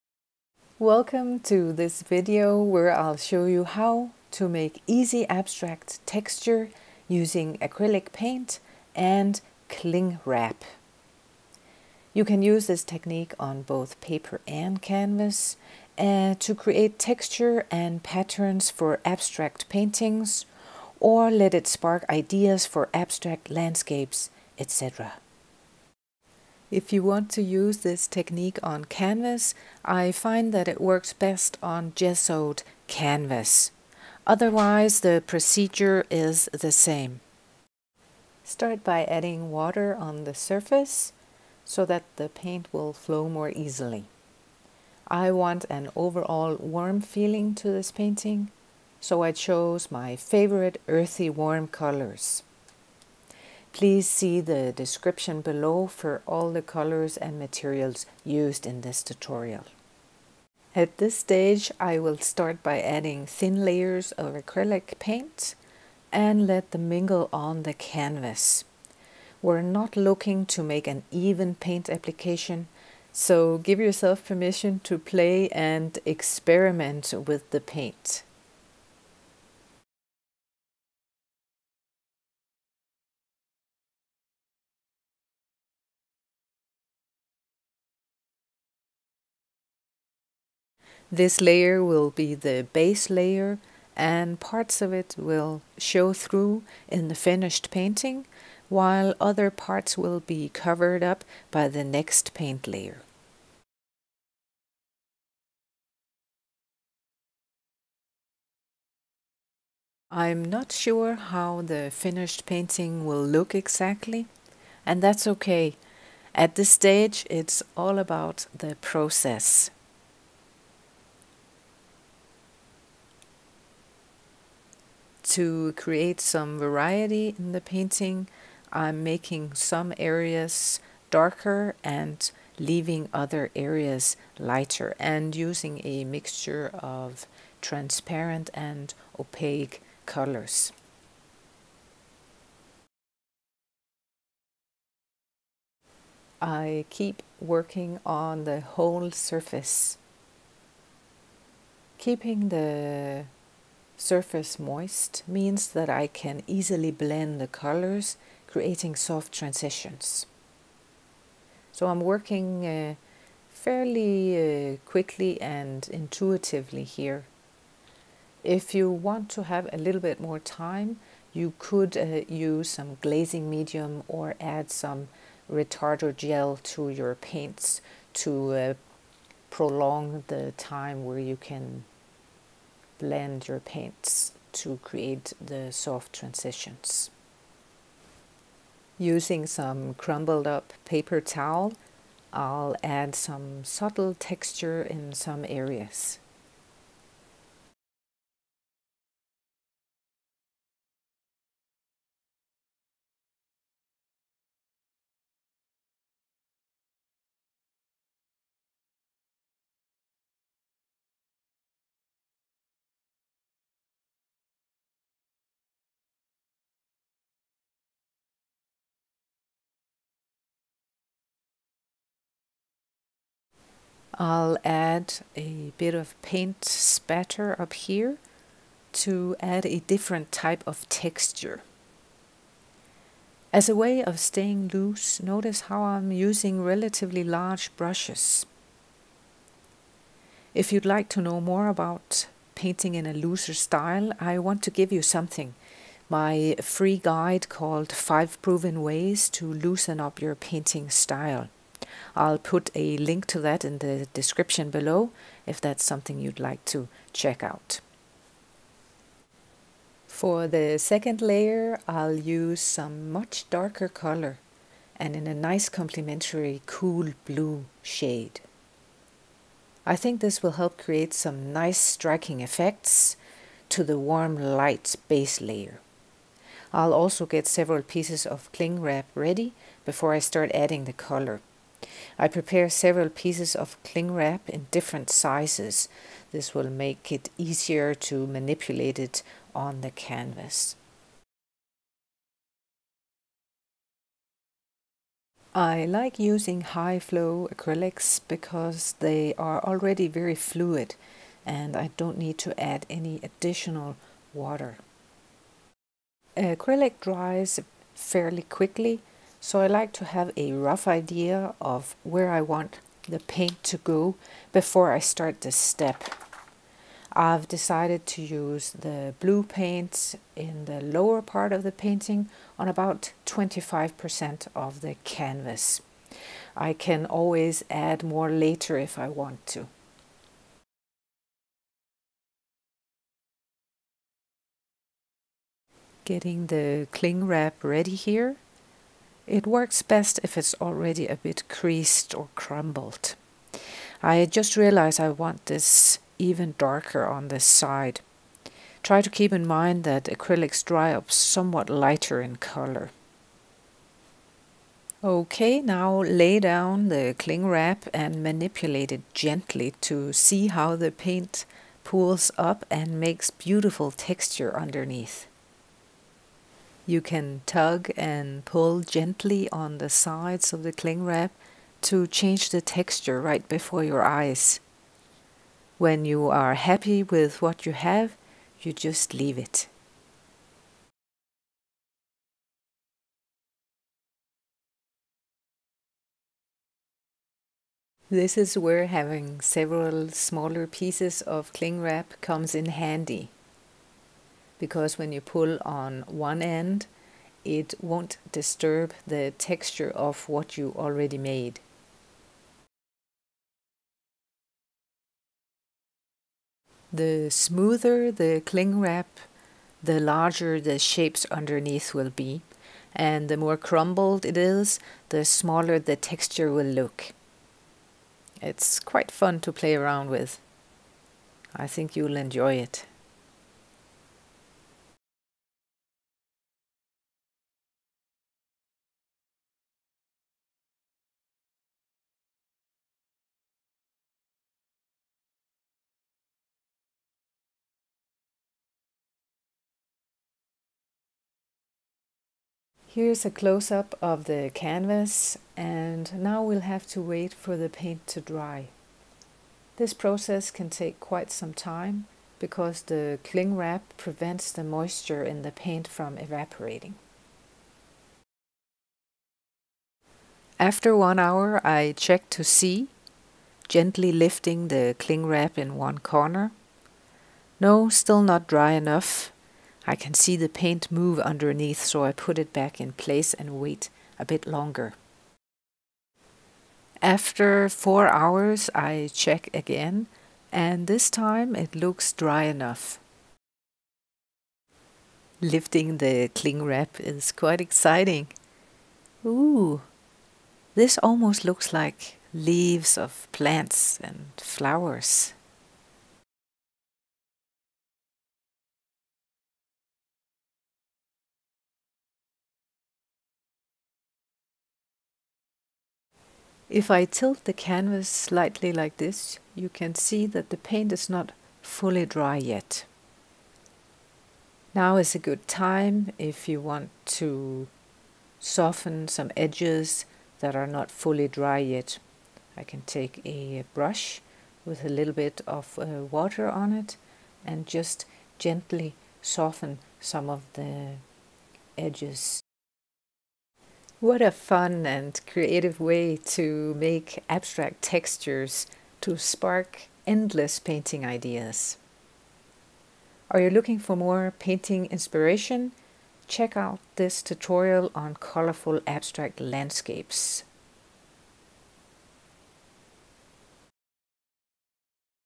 Abstract TEXTURE for Endless Painting ideas I Acrylics
►► Get my FREE GUIDE - 5 Proven Ways To Loosen Up Your Painting Style

This is a video to show you how to make easy abstract texture using acrylic paint and cling wrap. This technique can be used on both paper and canvas. Use this texture to create beautiful abstract paintings or as accents in your more realistic paintings.

Materials in the video:
Canvas coated with gesso or use thick watercolor paper (no need to gesso paper)
Acrylic colors:
Yellow ochre, Burnt umber, Transparent Yellow iron oxide, Nickel Azo Gold, Indigo blue, Ultramarine blue.
Cling Wrap
Spray bottle with water
Paper towels
Various brushes

What a fun and creative way to make abstract texture and spark endless painting ideas.